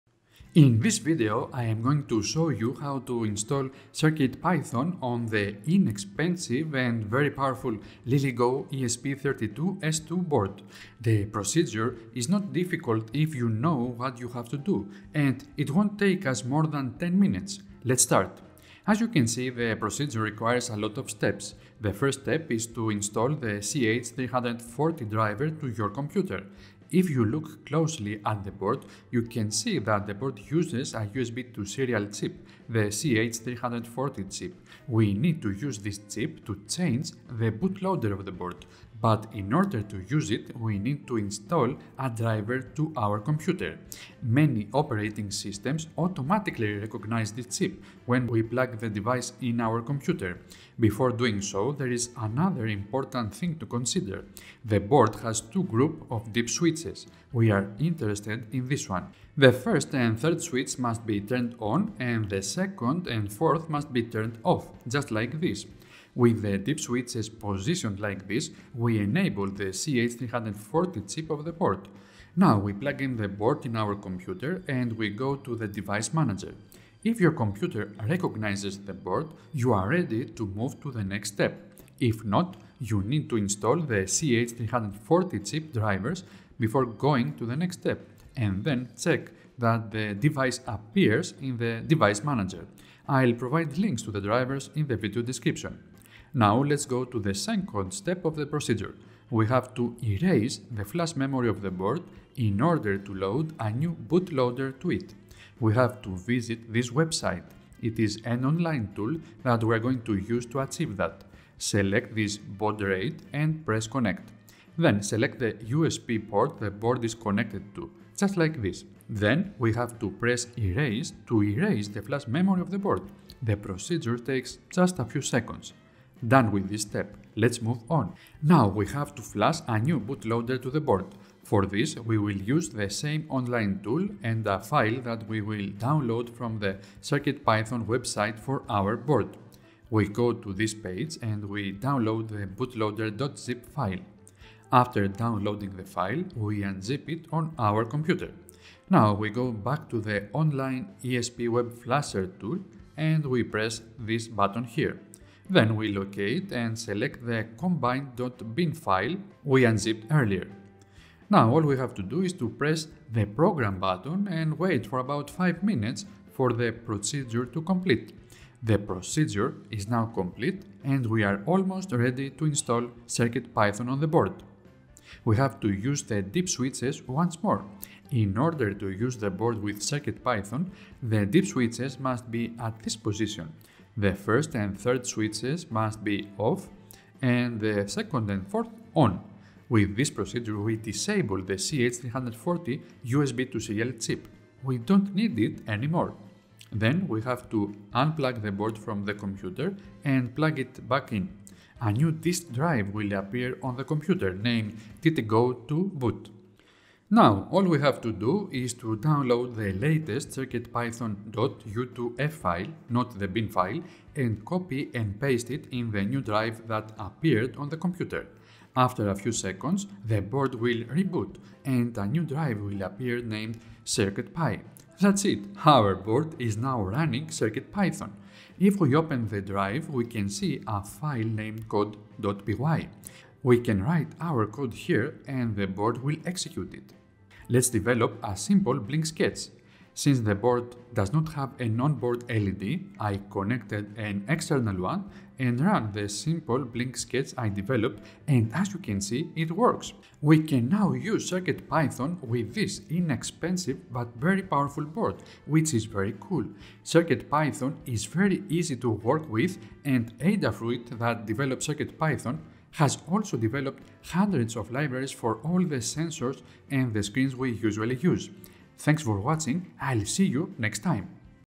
How to install CircuitPython on the Lilygo ESP32 S2 board.
The Lilygo ESP32 S2 board can run CircuitPython. In this video I am going to show you how to install CircuitPython on the inexpensive and very popular Lilygo ESP32 S2 board. The procedure is not difficult if you know what you have to do, and it won’t take us more than 10 minutes. Let’s start.

As you can see the procedure requires a lot of steps. The first step is to install the CH340 driver to your computer. If you look closely at the board, you can see that the board uses a USB to Serial chip, the CH340 chip. We need to use this chip to change the bootloader of the board, but in order to use it we need to install a driver to our computer. Many operating systems automatically recognize this chip when we plug the device in our computer.

 Before doing so, there is another important thing to consider. The board has 2 groups of dip switches. We are interested in this one.

The first and 3d switch must be turned on and the 2nd and 4th must be turned off, just like this. With the dip switches positioned like this we enable the CH340 chip of the board. Now we plug in the board in our computer and we go to the device manager. If your computer recognizes the board, you are ready to move to the next step. If not, you need to install the CH340 chip drivers before going to the next step and then check that the device appears in the device manager. I will provide links to the drivers in the video description